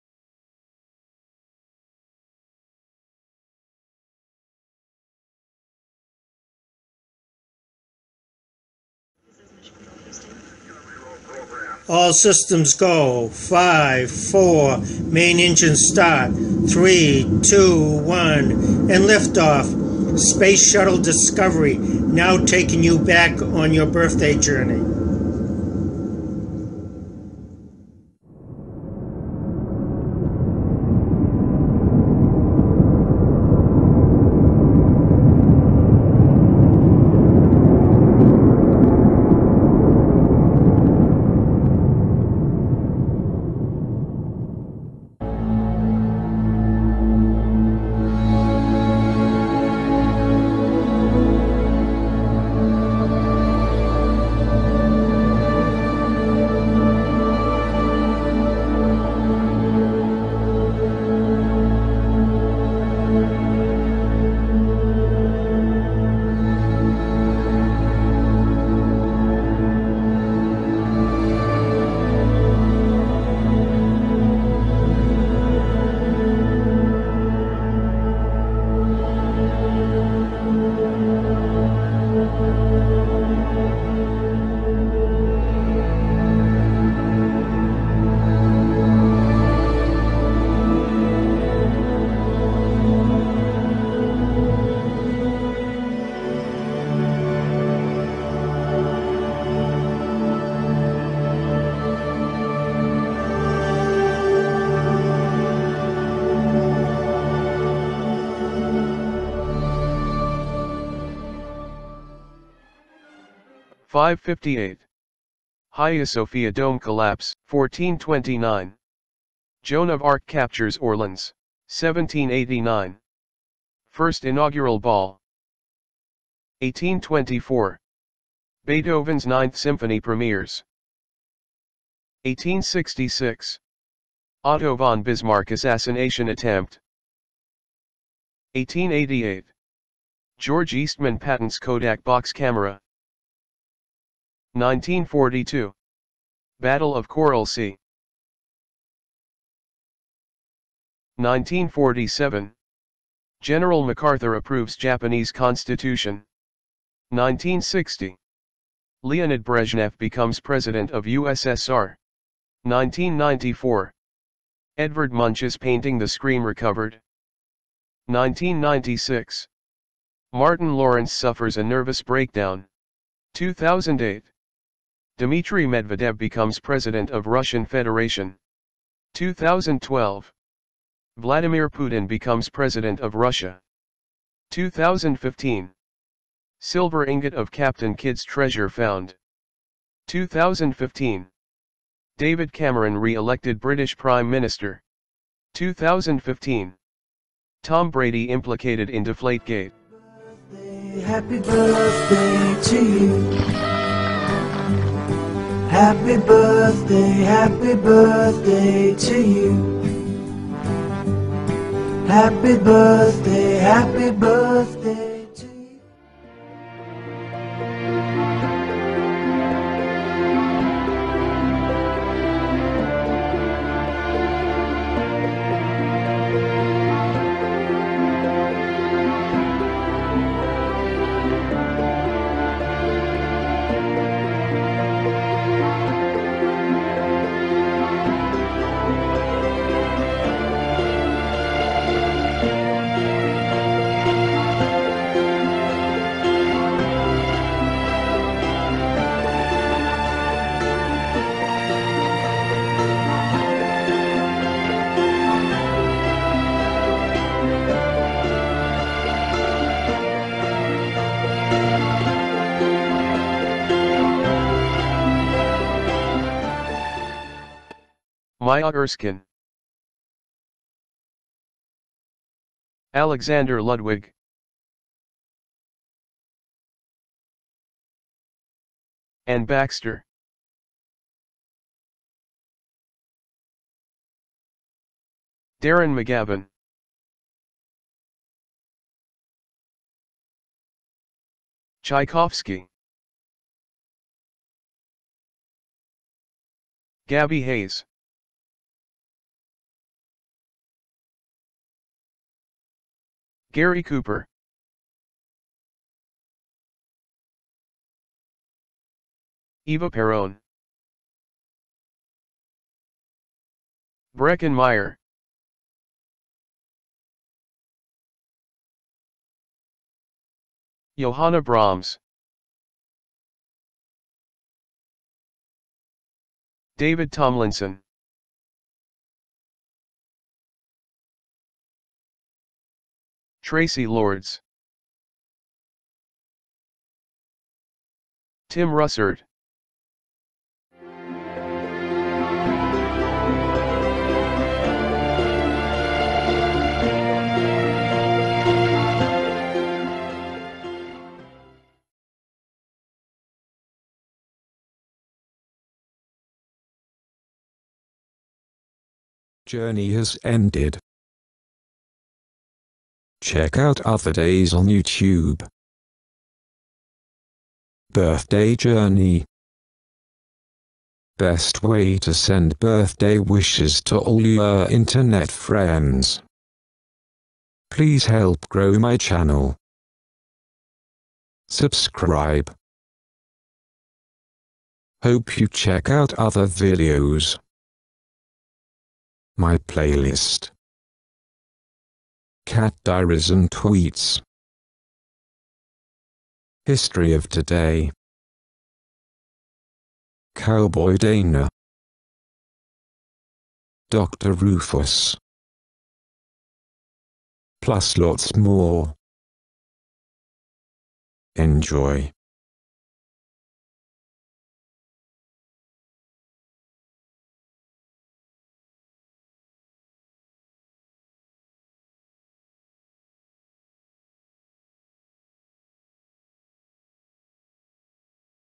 Birthday Journey May 7 (voice)
Birthday Message--Celebrate your Birthday May 7 through time. Journey through historical events, see your horoscope and famous people who share your Birthday day. GREAT way to send Birthday wishes to all your internet Friends! Easy and FREE-just copy and paste